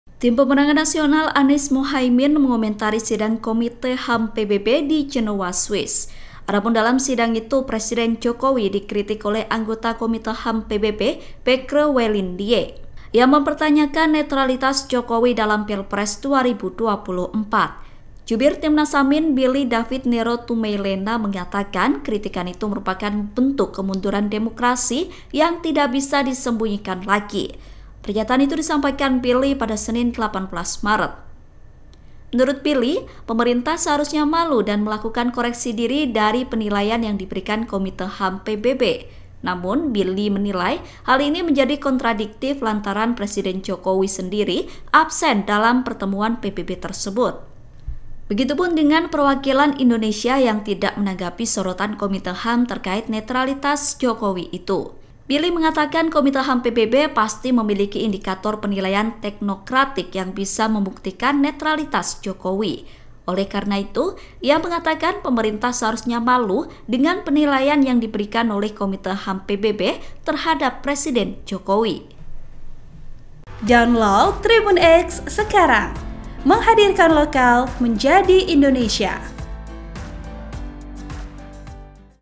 Jokowi Dikritik Komite HAM PBB soal Netralitas Pilpres Kemenlu cuma Diam, Timnas AMIN: Harusnya Malu
TRIBUN-VIDEO.COM - Tim Pemenangan Nasional Anies-Muhaimin (Timnas AMIN) mengomentari Sidang Komite HAM PBB di Jenewa Swiss.

Adapun dalam sidang itu, Presiden Jokowi dikritik oleh Anggota Komite HAM PBB Bacre Waly Ndiaye.

VP: Irvan